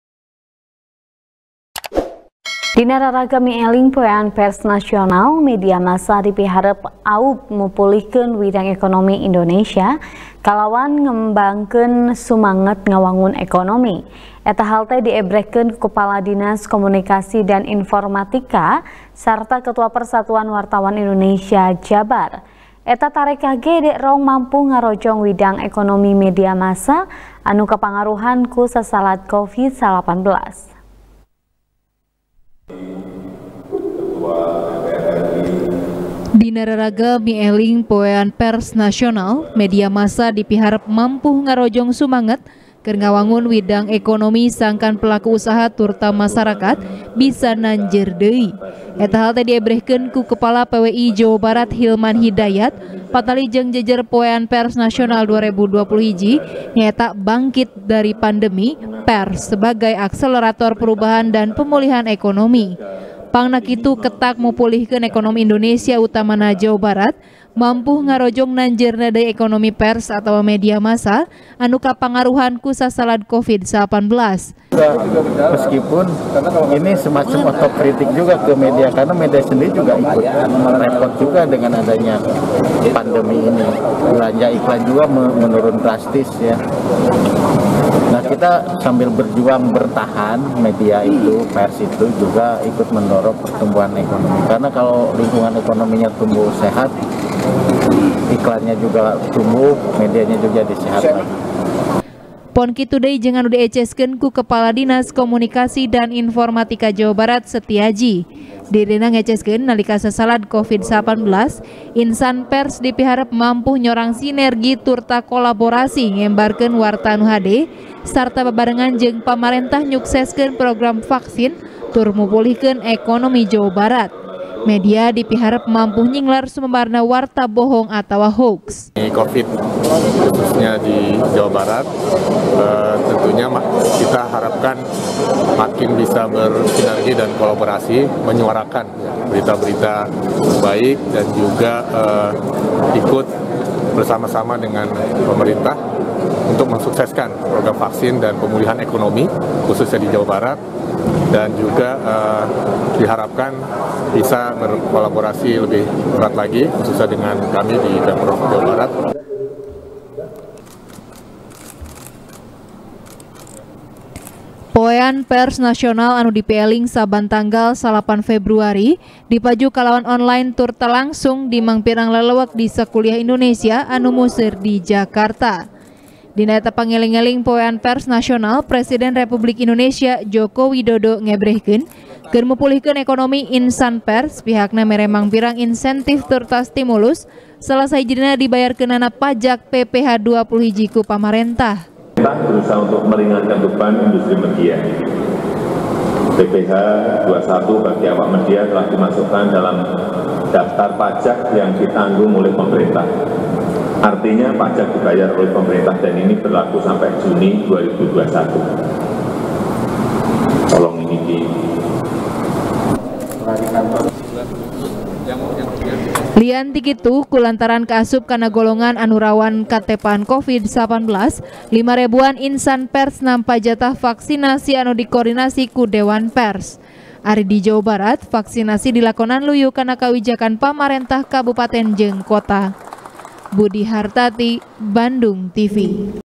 Poean Pers Nasional, Media Massa Dipiharep Aub Mupulihkeun Ekonomi | TP | BANDUNGTV
TP | BANDUNGTV | ASTRI CAHYANI | Dina Raraga Mieling Poean Pers Nasional, Media Massa Dipiharep Aub Mipulihkeun Widang Ekonomi Indonesia, Kalawan Ngembarkeun Sumanget Ngawangun Ekonomi. Eta Hal Teh Diebrehkeun Ku Kepala Dinas Komunikasi Dan Informatika Sarta Ketua Persatuan Wartawan Indonesia Jabar. Eta Tarekah Ge Dierong Mampuh Ngarojong Widang Ekonomi Media Massa, Anu Kapangaruhan Ku Sasalad Covid-19.